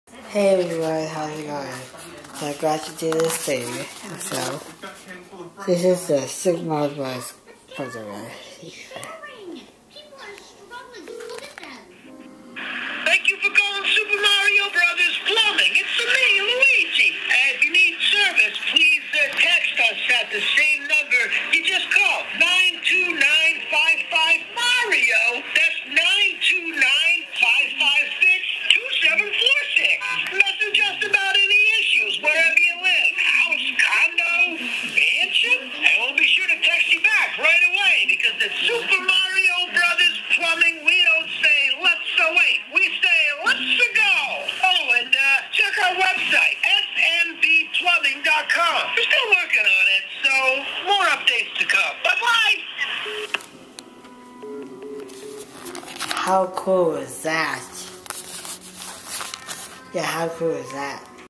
Super Mario Movie Phone Number Call
This is the video of me doing the phone call from the Super Mario Bros Movie plumbing commercial.